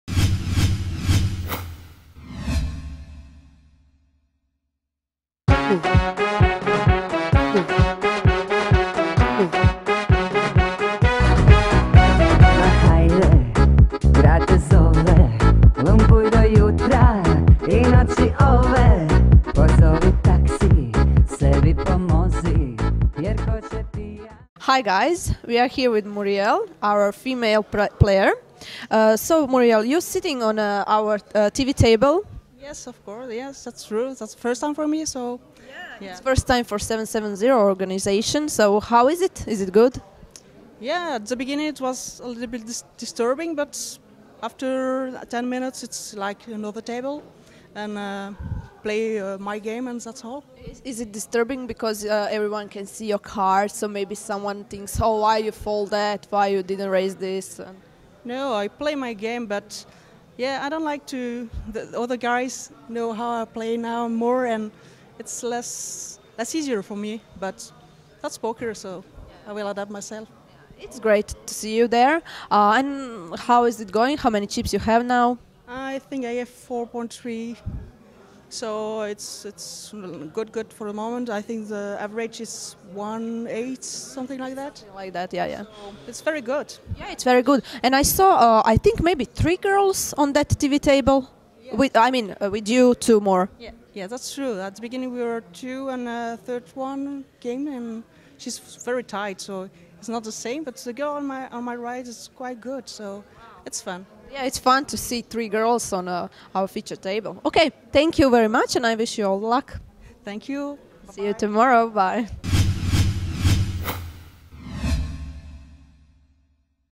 MegaPokerSeries Croatia 2012 - Interview Muriel Gomez Aragon [ENG]
Interview: Sonja Kovac
Réalisation: Nicolas Deru